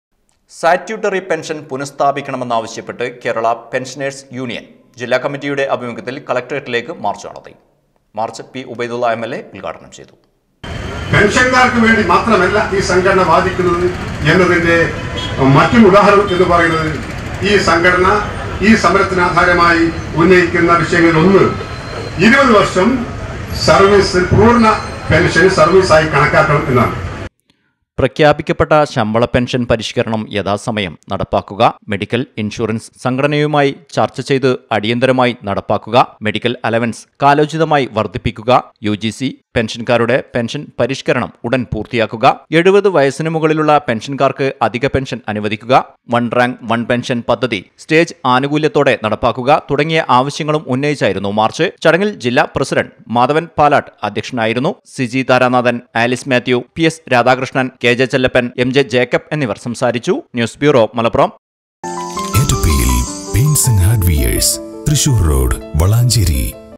കേരള പെന്‍ഷനഴ്‌സ് യൂണിയന്‍ ജില്ലാ കമ്മിറ്റിയുടെ ആഭിമുഖ്യത്തില്‍ കലക്ട്രേറ്റിലേക്ക് മാര്‍ച്ച് നടത്തി.
വിവിധ ആവശ്യങ്ങൾ ഉന്നയിച്ച്  കേരള പെന്‍ഷനഴ്‌സ് യൂണിയന്‍ ജില്ലാ കമ്മിറ്റിയുടെ ആഭിമുഖ്യത്തില്‍ കലക്ട്രേറ്റിലേക്ക് മാര്‍ച്ച് നടത്തി.